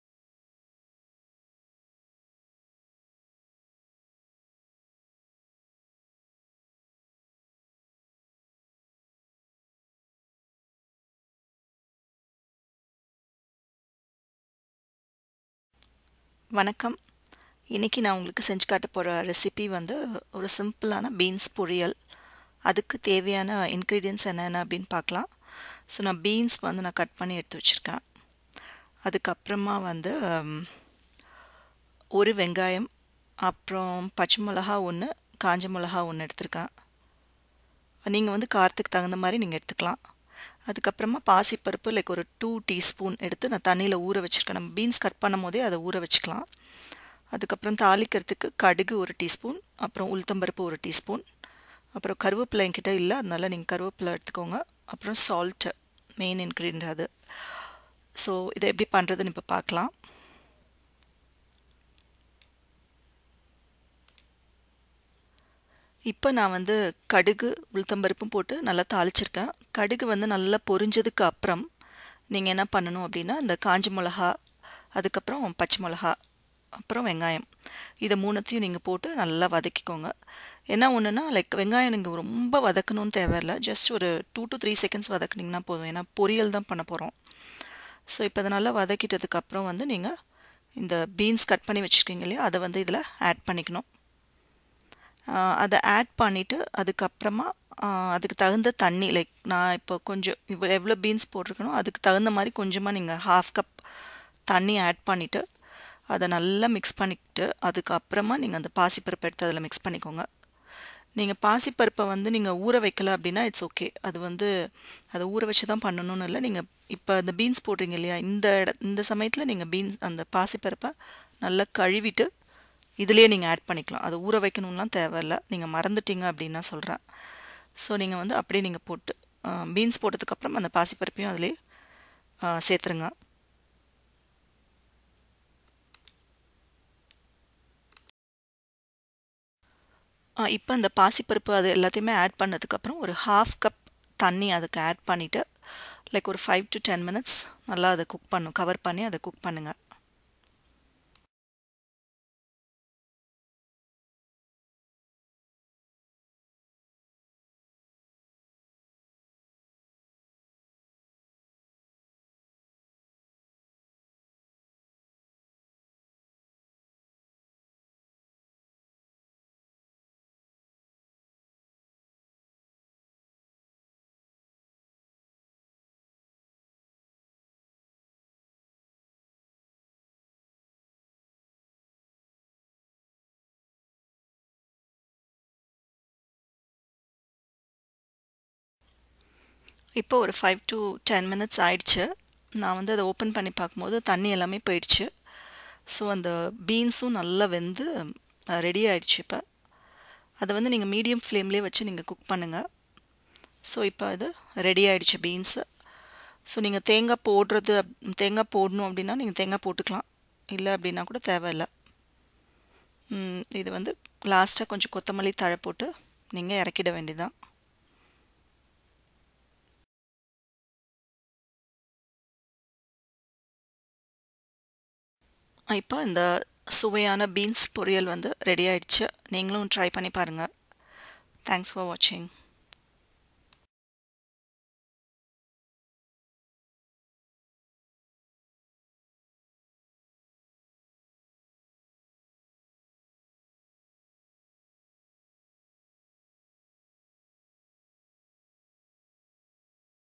beans poriyal in tamil / பின்ஸ் பொறியல்
This video takes you through the procedure of making very simple  and quick beans poriyal /subji / palya.
Ingredients :
1. beans - 10-20
2.onion - 1
3.green chillie - 1-2(according to your spice level)
4.red chillies - 2
5.moong dhal - soak for 10-15 min
6.salt - according your taste
7. oil for cooking
8. mustard - 1/2 tsp
9.Black gram dhal - 1/2 tsp
10 curry leaves - 1 sprig.
Method:
step 1. Heat oil in a pan,once oil is heated add mustard and black gram dhal.
step 2. Add green chillies,red chillies, curry leaves and onion.
step 3. once the onion are added saute it for just 2-3 secs and then add the beans.
step 4. Add1/2 cup water , salt and add the moong dhal.
step 5. cover and cook it for 5-10 min or until the beans are boiled well.(optional u can add 1tsp of grated coconut)